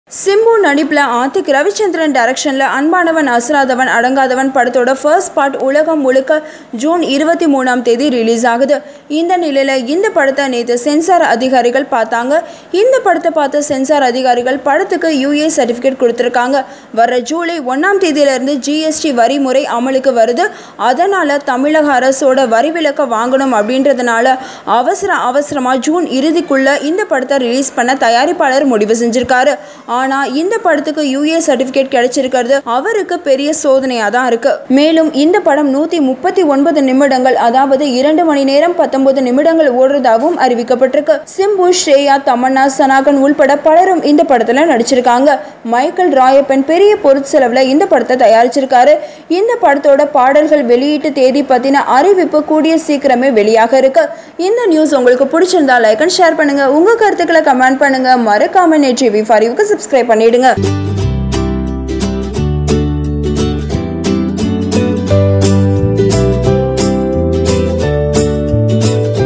AAA Gets UA Certificate From The Censor Board | கிடைத்ததனால் மனமுடைந்த இயக்குனர்
Simbu, the youth icon is busy with his upcoming project, Anbanavan Asaradhavan Adangadhavan. The film has been shot as AAA1 and AAA2. The first part will be released on 23rd June 2017 and the next part will be released in December.
 The film has Shriya Saran, Tamannaah Bhatia and Neetu Chandra. Yuvan Shankar Raja composed the music.
The film is directed by Adhik Ravichandran of Trisha Illana Nayanthara fame. The film has been certified as UA. Adhik Ravichandran felt disappointed that his film has been given UA.